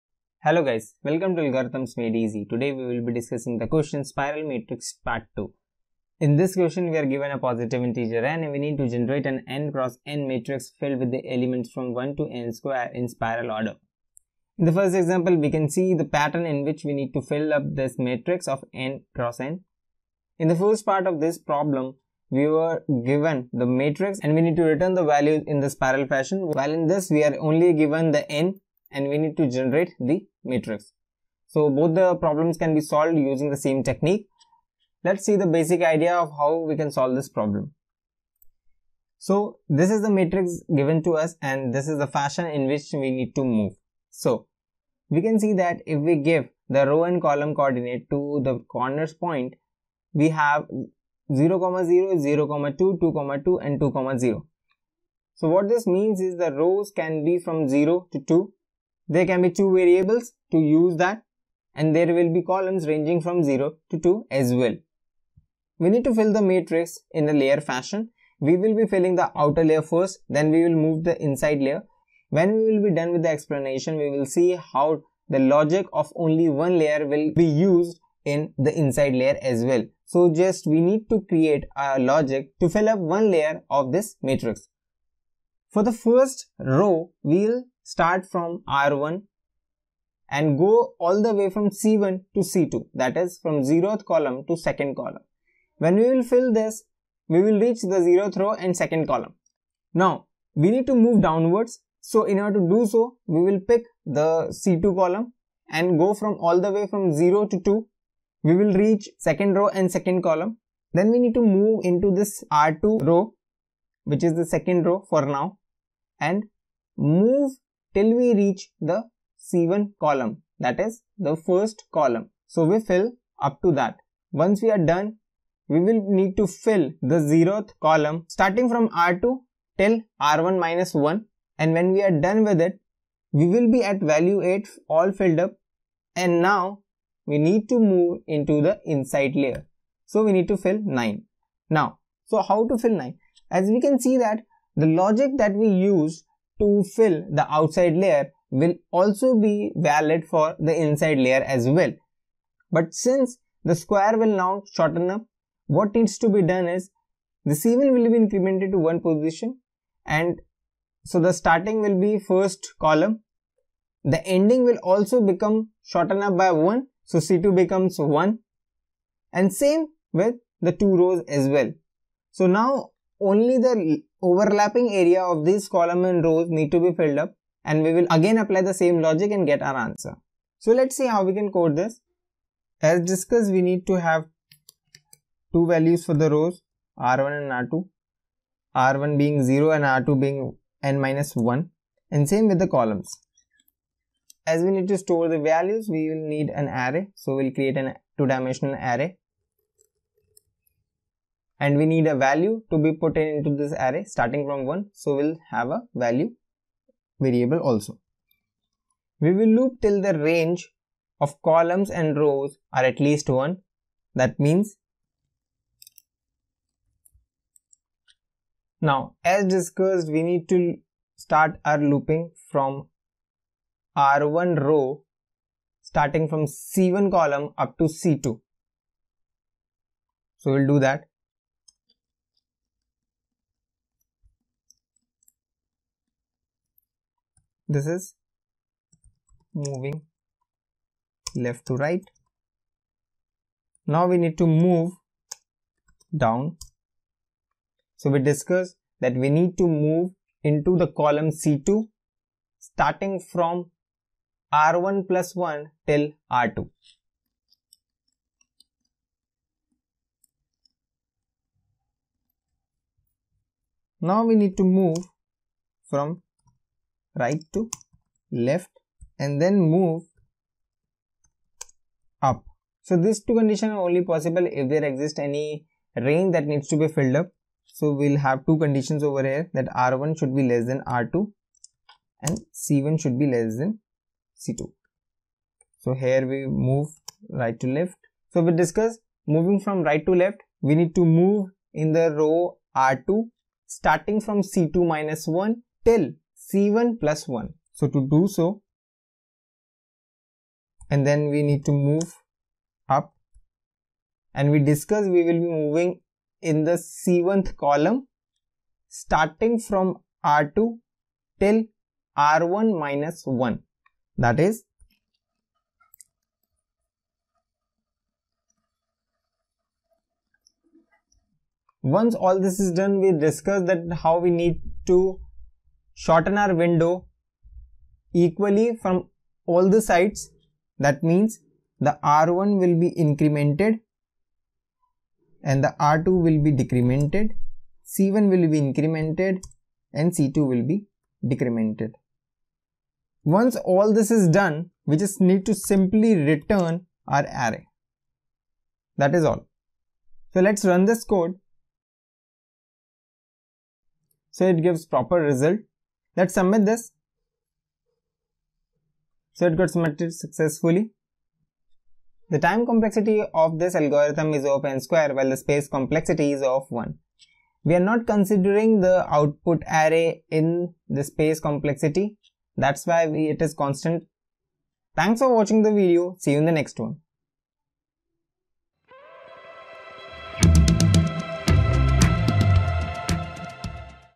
Spiral Matrix II | Live Coding with Explanation | Leetcode - 59
Detailed explanation for Spiral matrix 2 leetcode problem 59.

UPI: algorithmsmadeeasy@icici